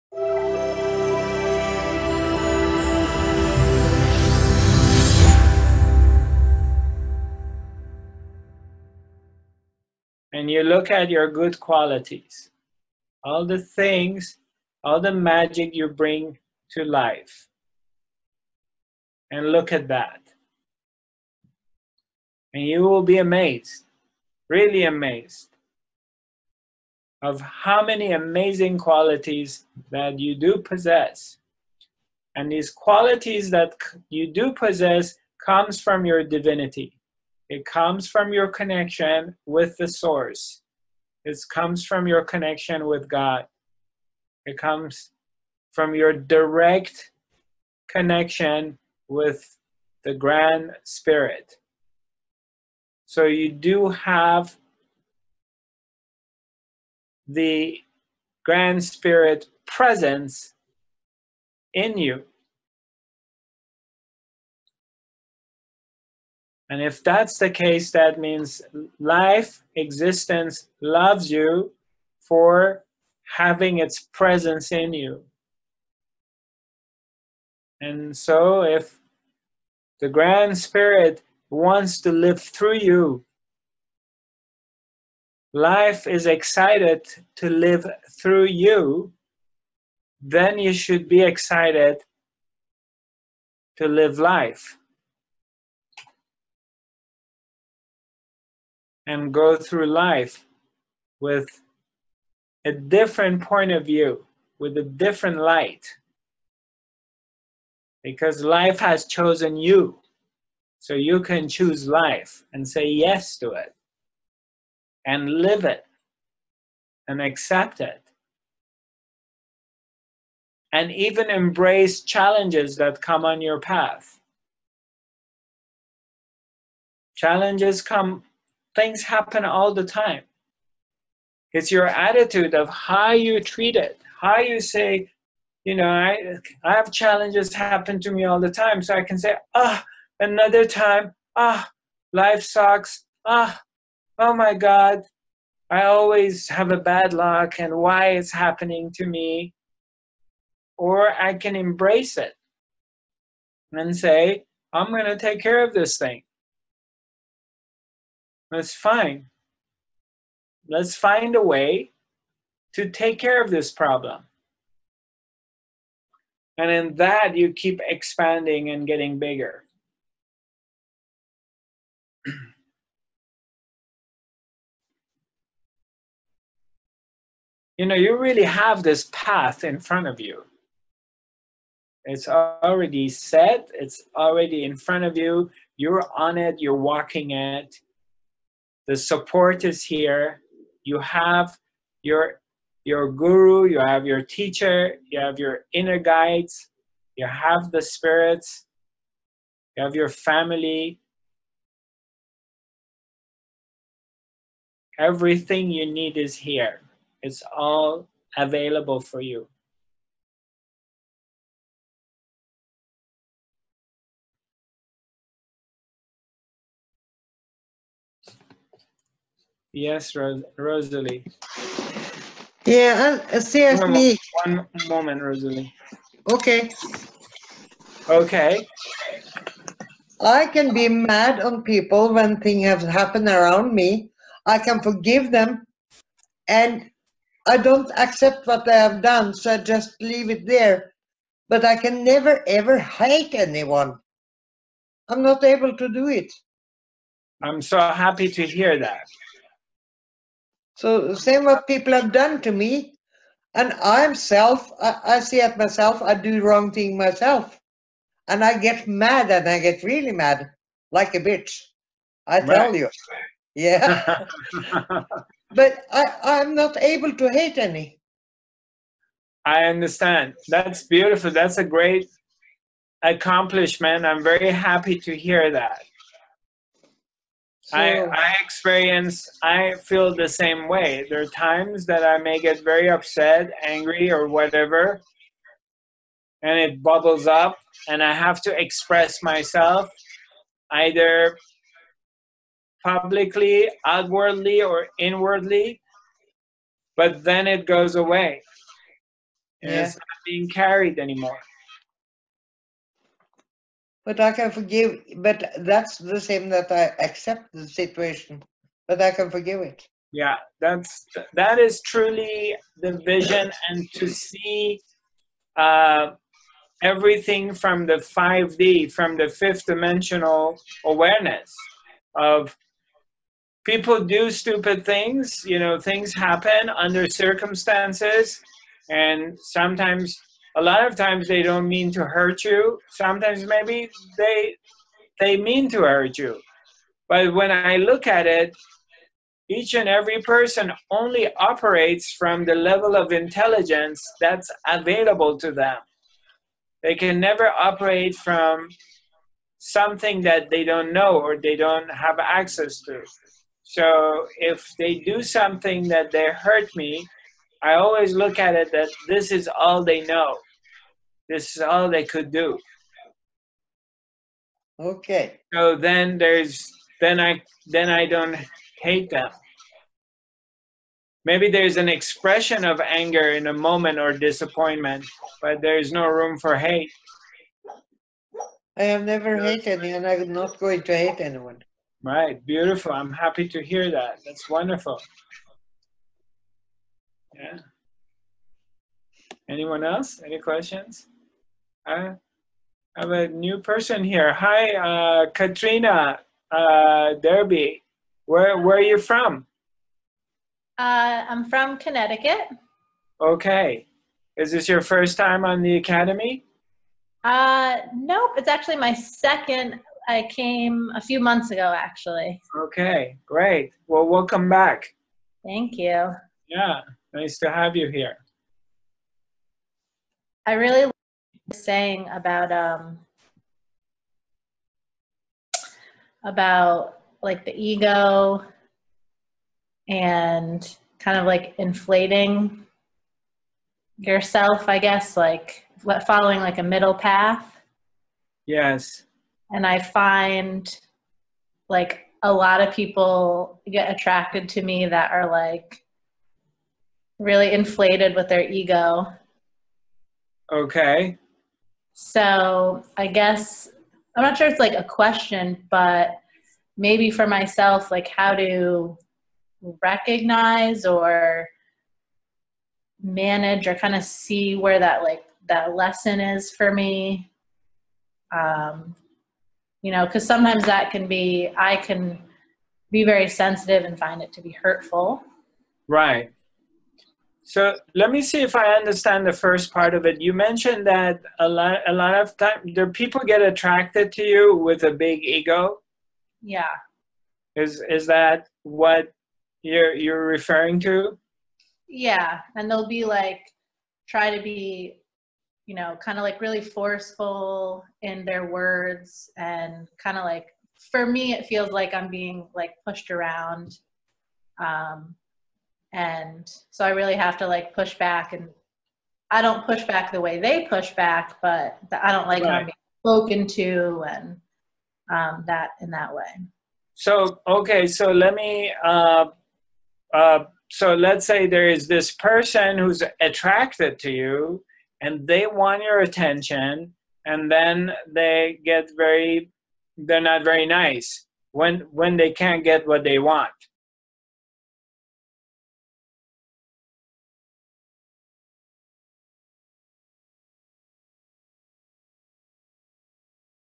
Spiritual Teacher Zarathustra giving clues of how to be Here & Now - Part 03
By listening to his discourses and applying the insights and meditations suggested; you will be able to rapidly over come anxiety, depression and discover inner peace and inner silence and reach a balanced emotional state which leads to enlightenment, the ultimate happiness.